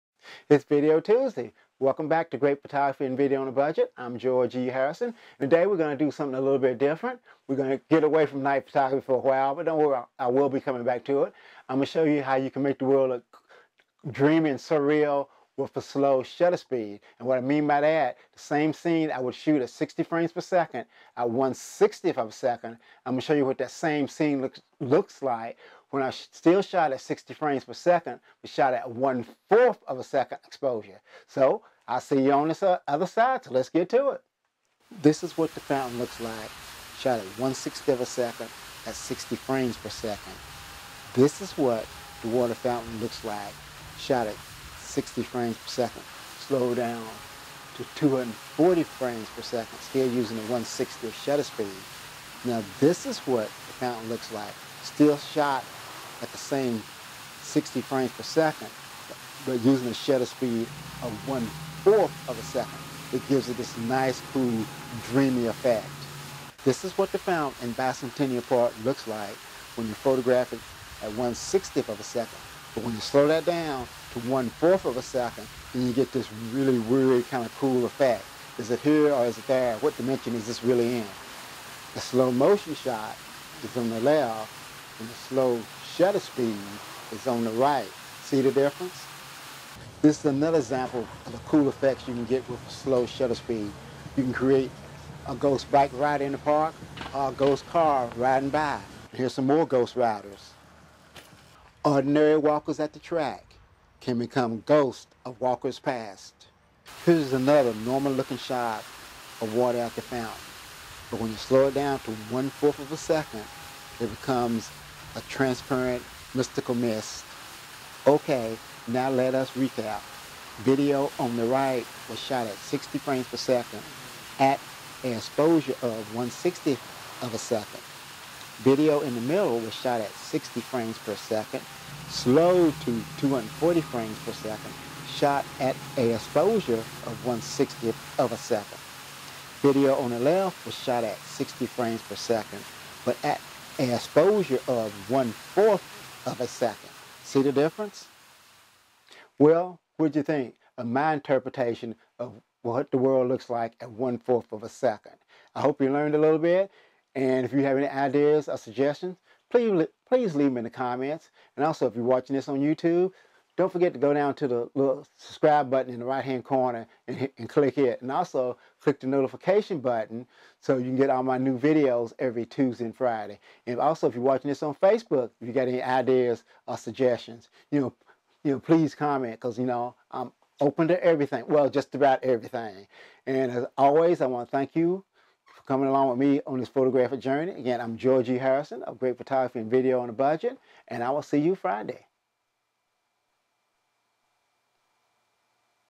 Not Slow Motion but a Slow Shutter Speed
Some cool effects you can get shooting video with a slow shutter speed.